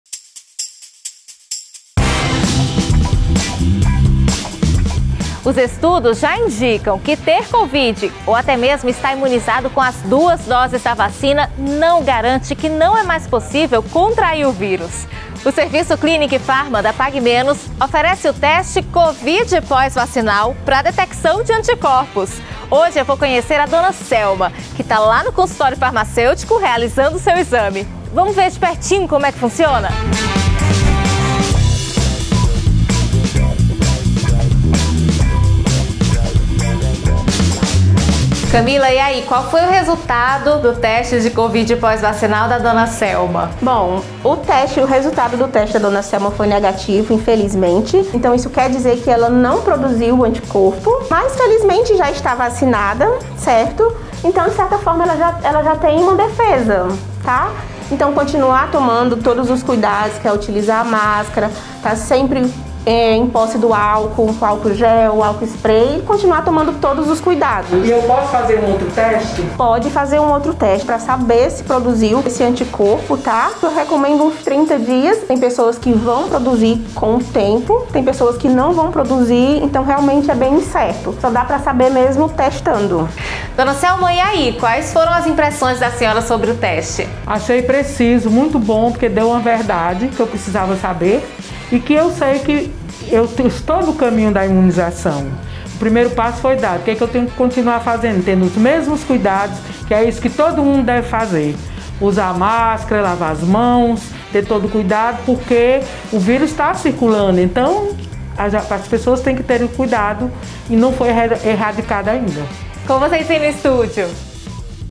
Pague Menos & Você: Teste de Anticorpos Neutralizantes de COVID-19
O teste rápido de anticorpo neutralizante detecta a presença de anticorpos que bloqueiam a entrada da COVID-19 no organismo. Entenda mais no vídeo.